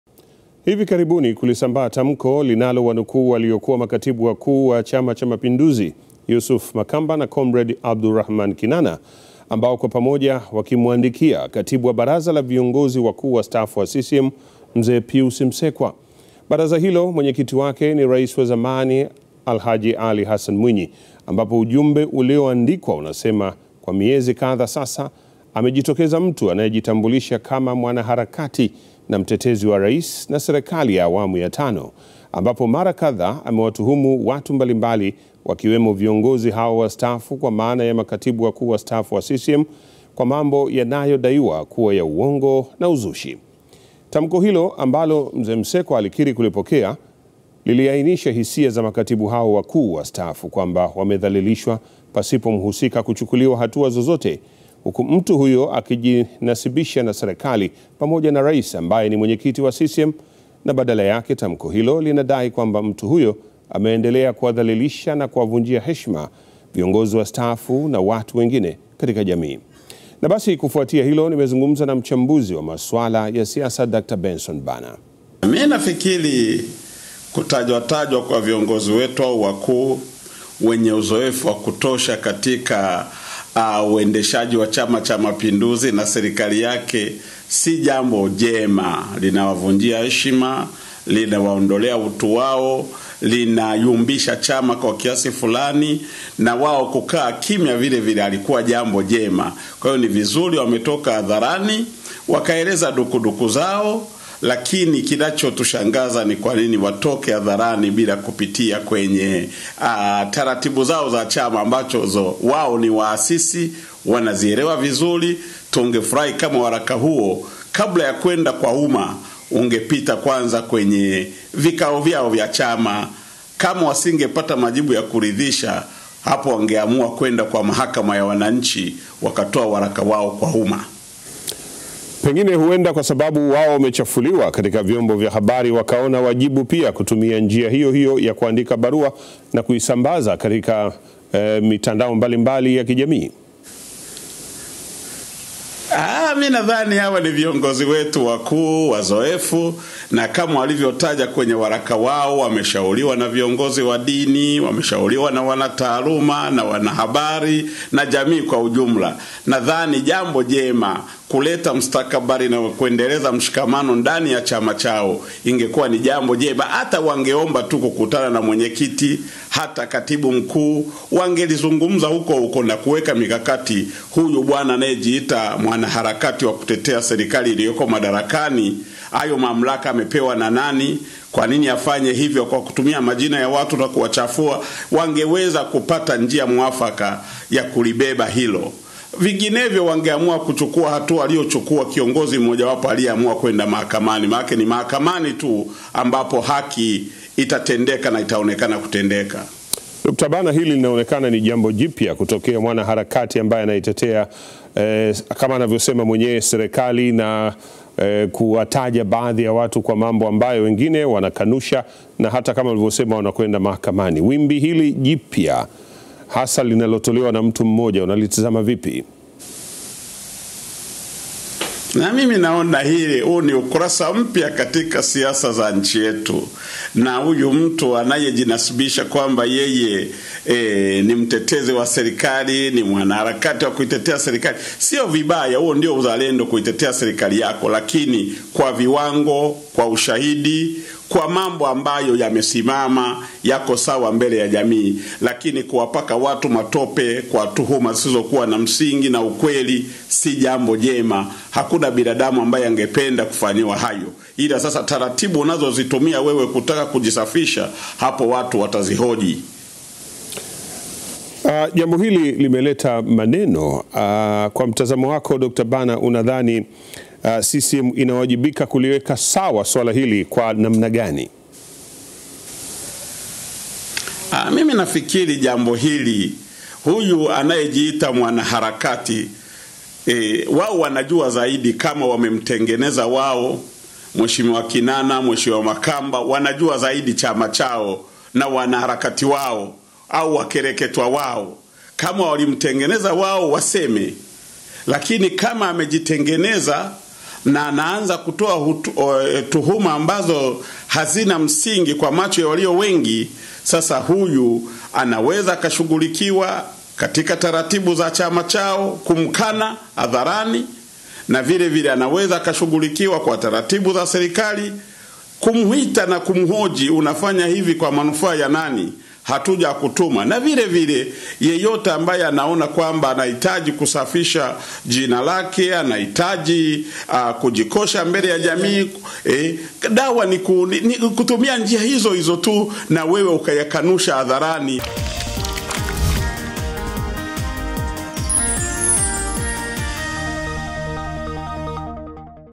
Sakata la Kinana na Makamba kuandika barua CCM kupinga kuchafuliwa
Tumezungumza na mtaalamu wa siasa na mchambuzi mwandamizi Benson Bana kuhusiana na barua iliyosambaa mitandaoni na kuthibitishwa na aliyekuwa Abdulrahman Kinana kuwa imeandikwa naye na aliyekuwa mtangulizi wake Yusuph Makamba wakitaka chama cha CCM kujadili suala la kuchafuliwa kwa viongozi wastaafu wa chama hicho na Cyprian Musiba.